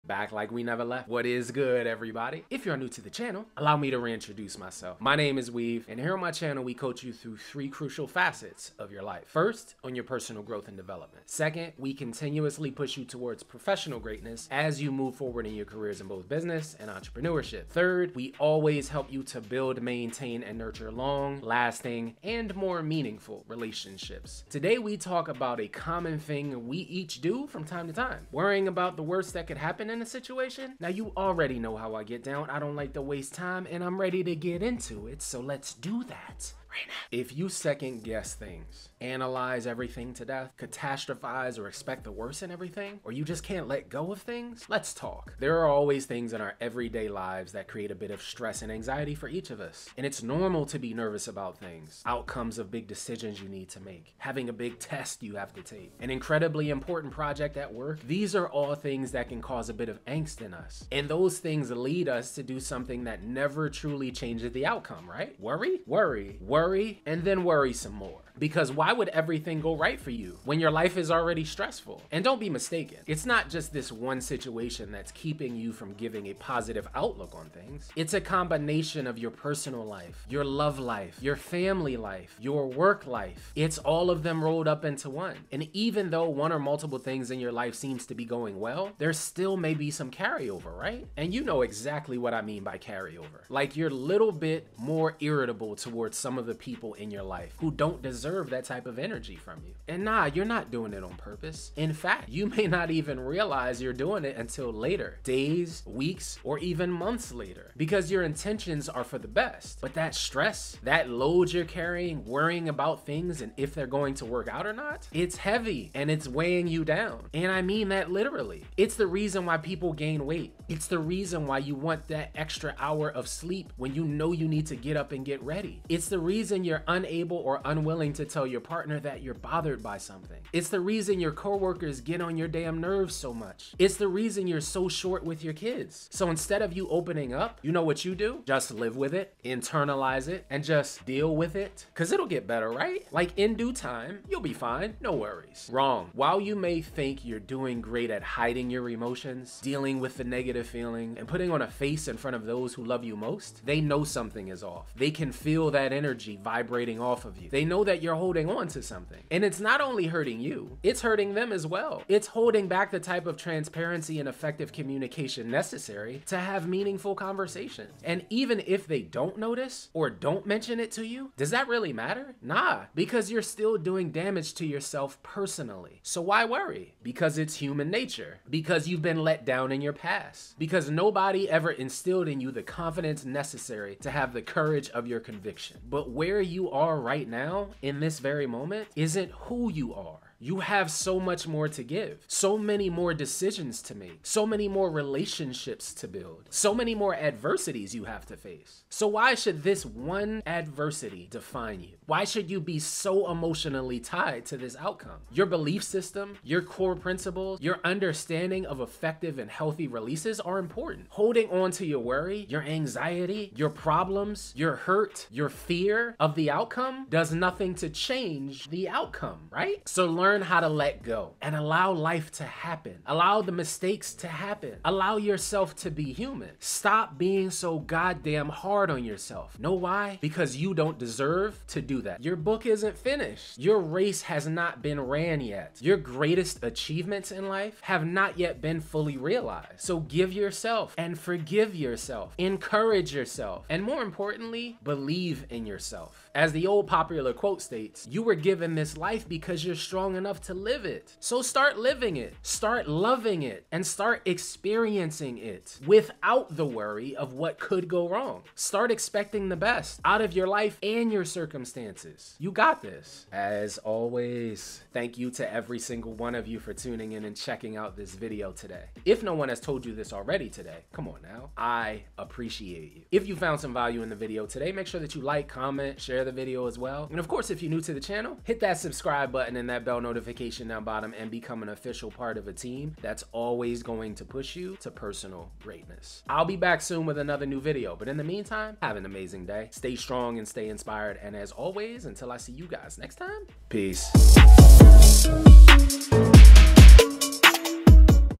WORRYING ABOUT IT WON'T CHANGE THE OUTCOME | How To Stop Worrying & Causing Yourself So Much Anxiety
How to stop worrying so much. The importance of worrying less and how to stop causing yourself to have anxiety about your life! Tap in!

Weav
Let’s Connect: 🔗

*All Interview, Collaborations, or Podcasts Requests: 🎙